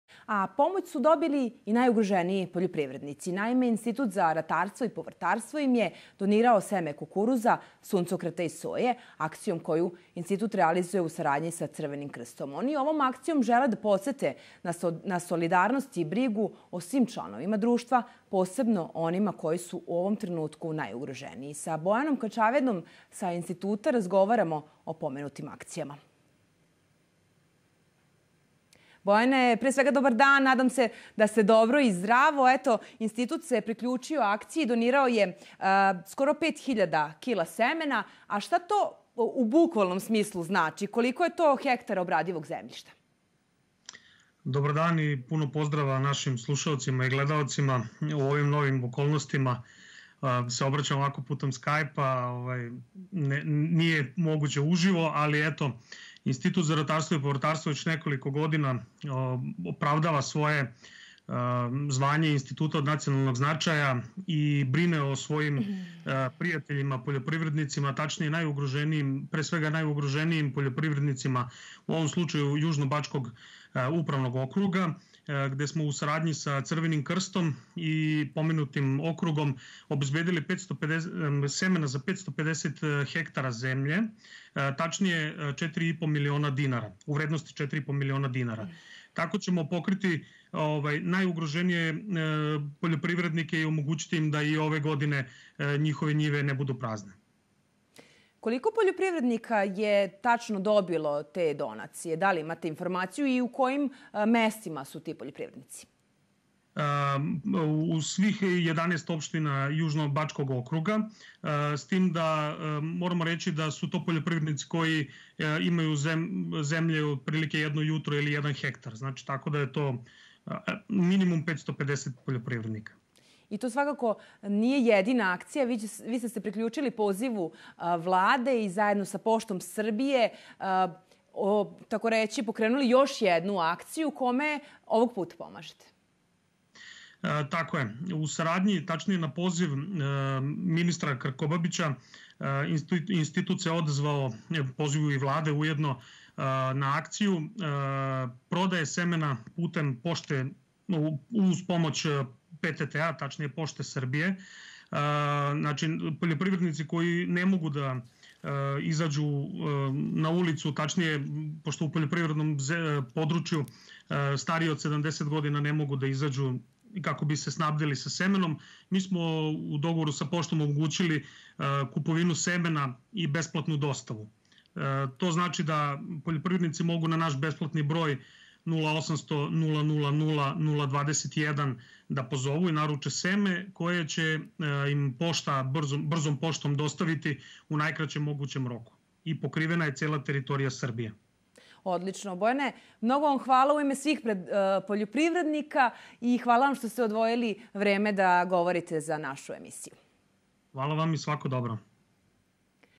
Pomoć su dobili i najugroženiji poljoprivrednici
Pomoć su dobili i najugroženiji poljoprivrednici. Naime, Institut za ratarstvo i povrtarstvo im je donirao seme kukuruza, suncokreta i soje. Akcijom, koju Institut realizuje u saradnji sa Crvenim krstom, žele da podsete na solidarnost i brigu o svim članovima društva, posebno o onima koji su u ovom trenutku ugroženi.